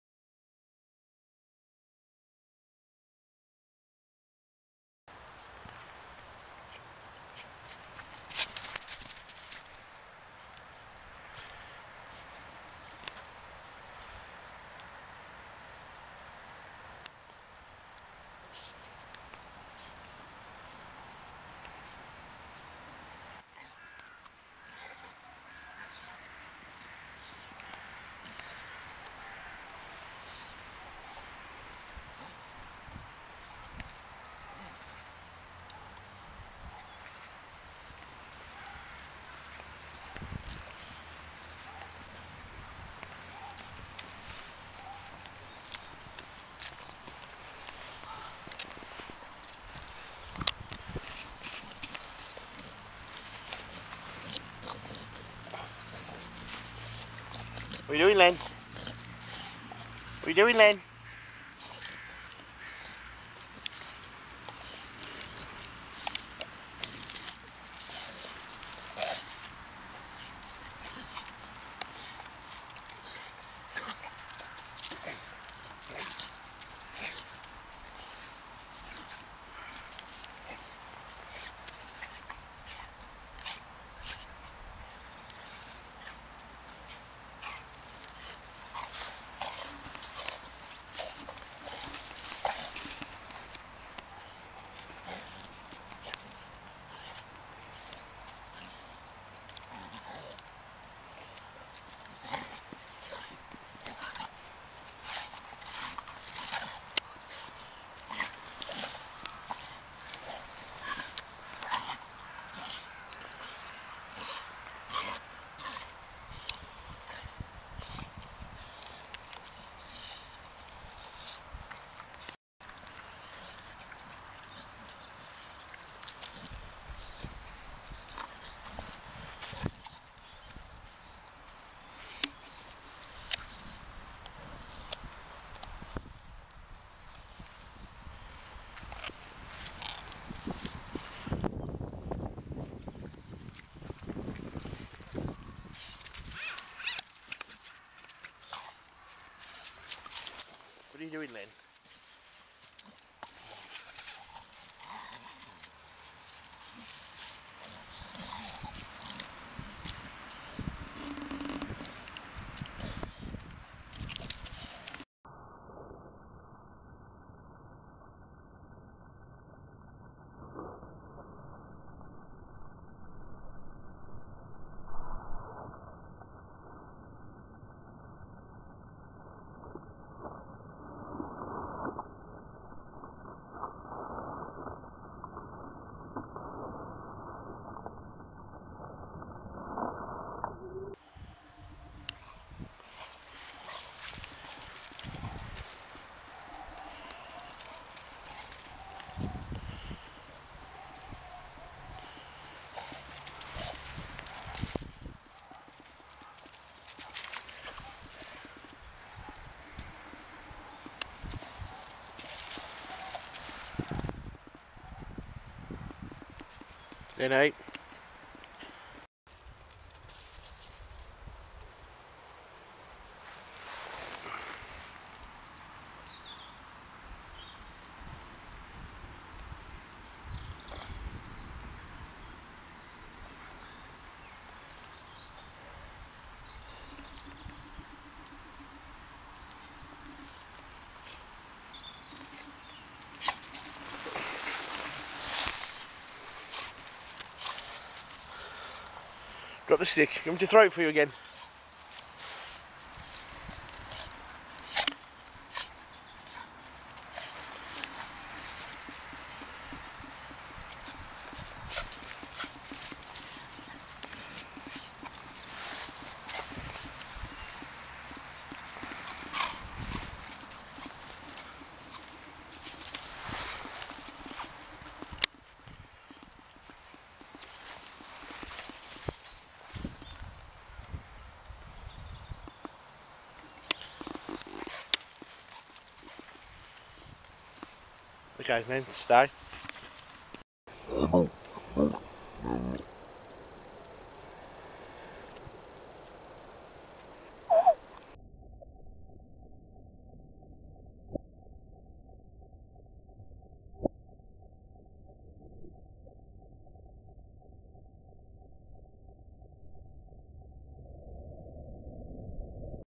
Boxing Day Lenny.wmv
Just an excuse for me to test out my Toshiba Camileo P30, by letting Lenny loose in Selly Oak park on Boxing day 2009.